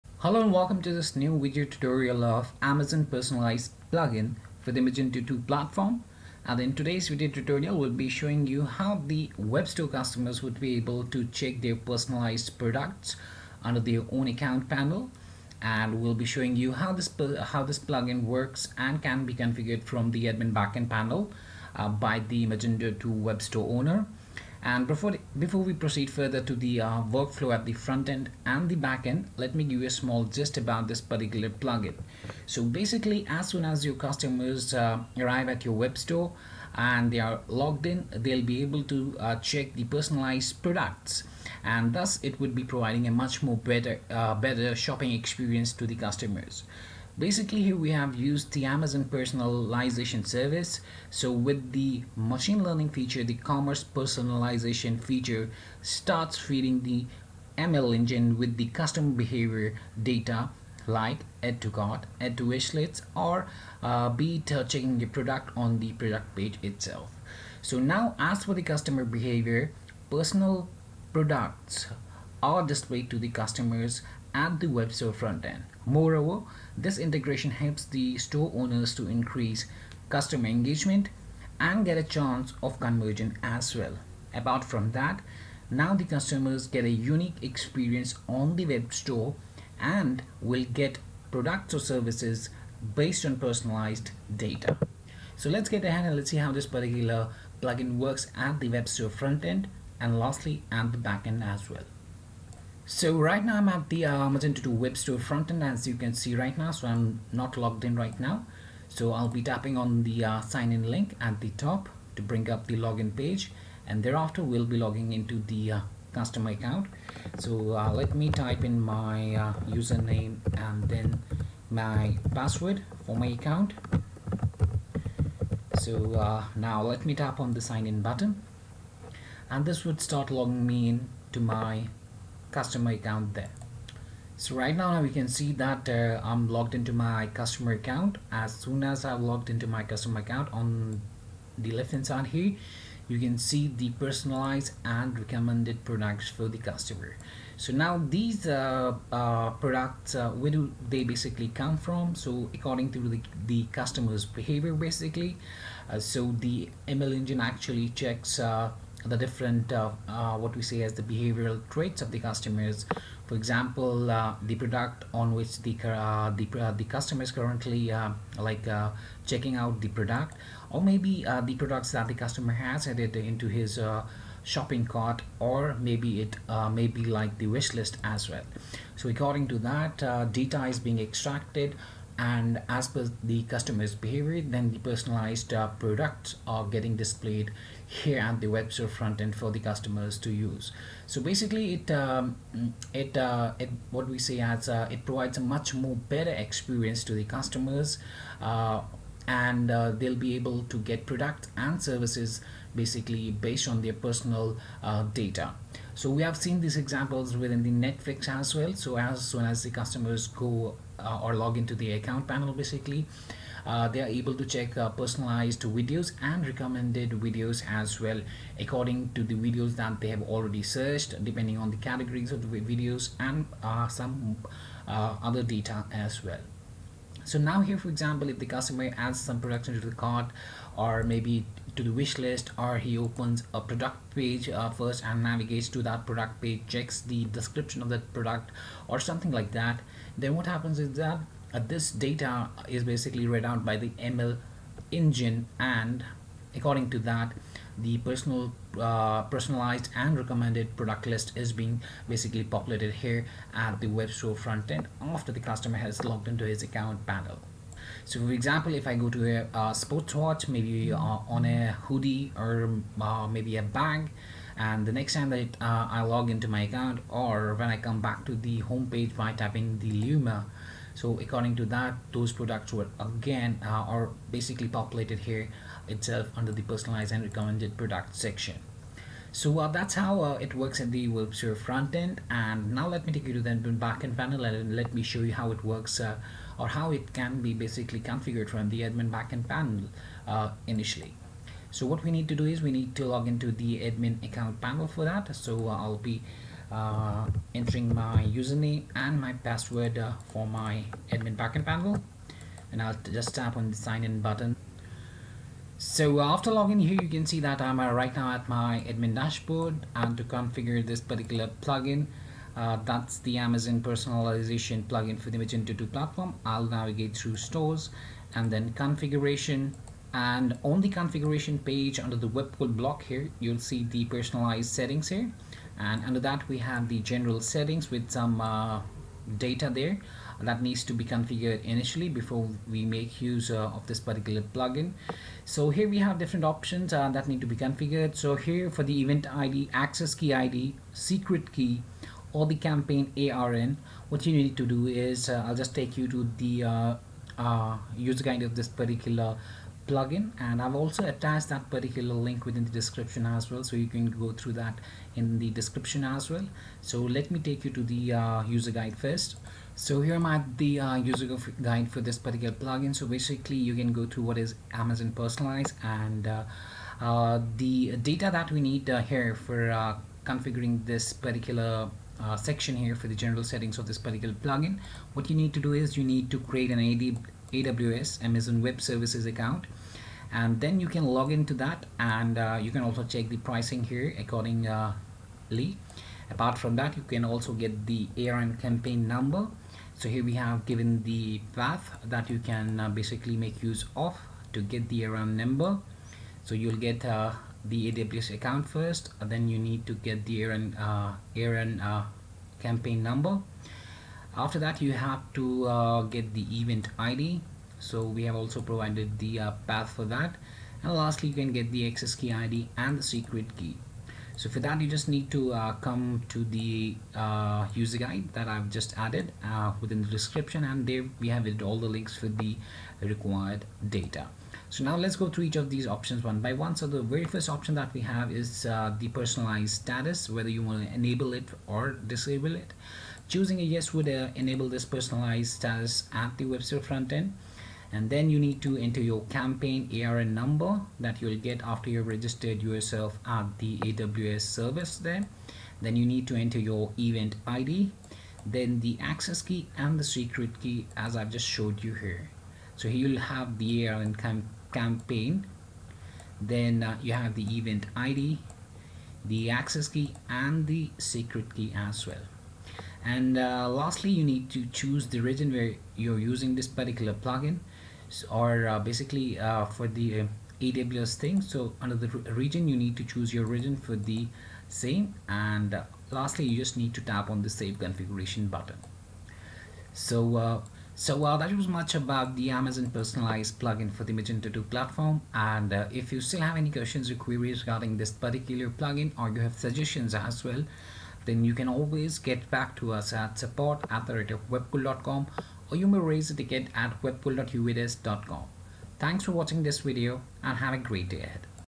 Magento 2 Amazon Personalize | Product Recommendation Plugin - Workflow
The extension integrates Amazon Personalize service into Magento 2 online stores to provide real-time personalization and machine learning recommendation. With this, every customer will have a unique experience on the web store and will view products based on personalized data.

2. Raise a Ticket via our HelpDesk system